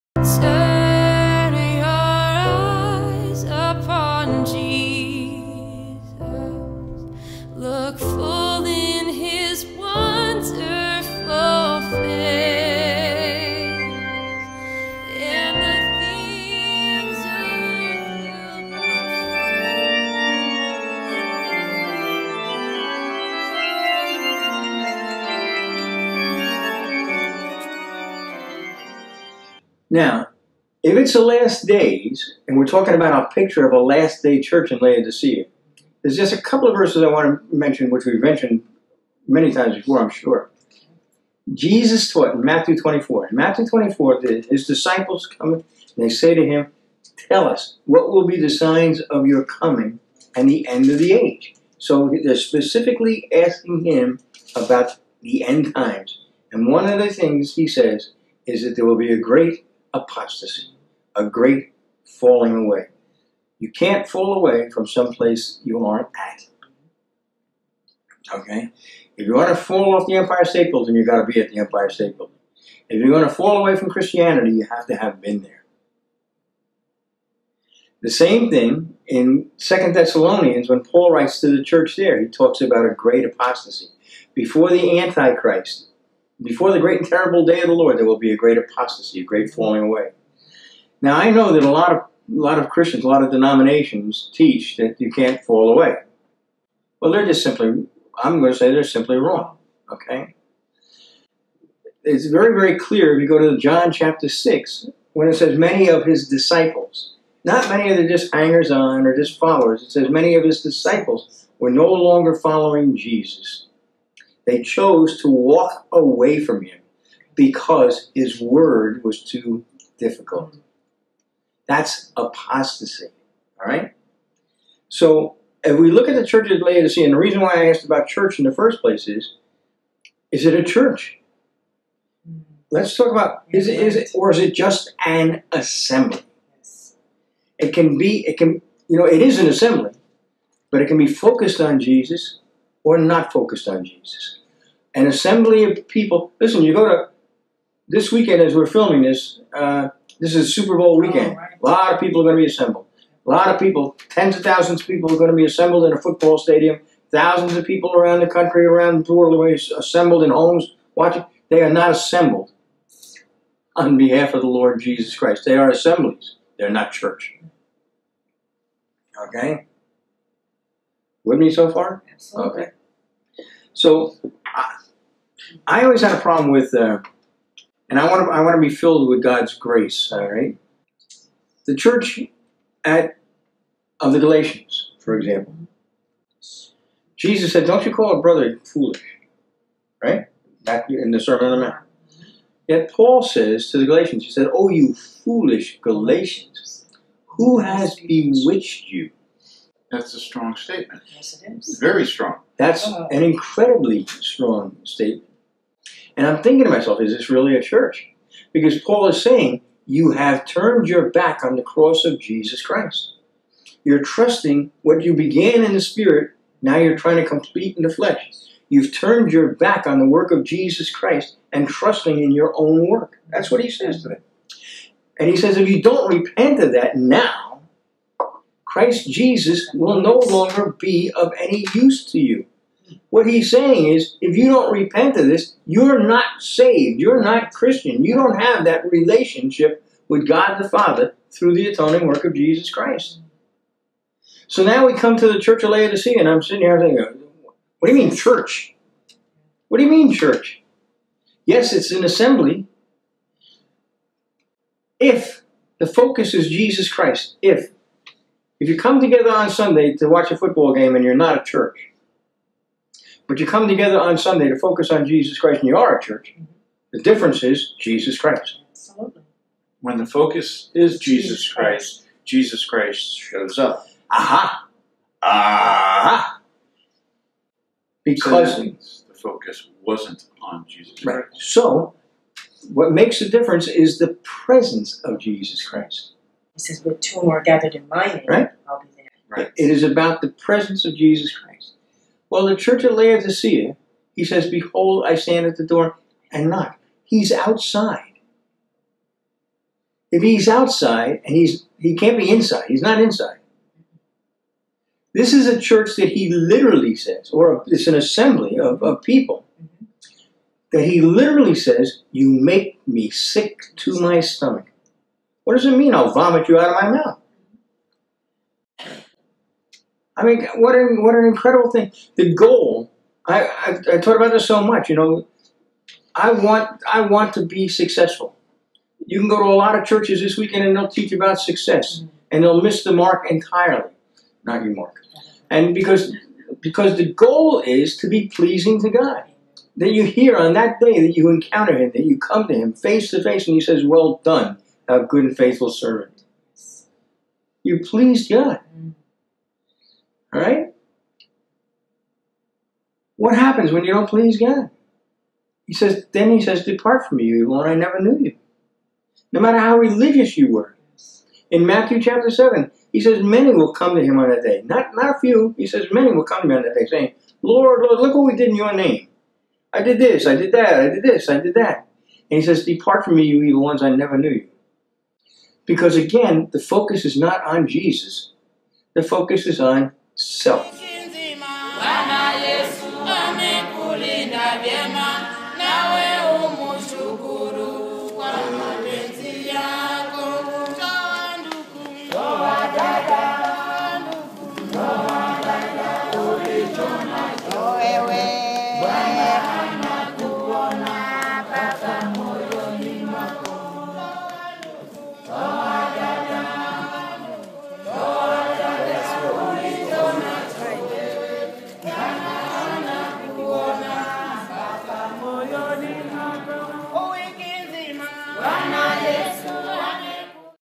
Church - Is It This Or Is It That?
Is it a building or a corporation? Is it an organization or is it an organism? Or, is it simply the presence of the Lord in two or more believers gathered in His name?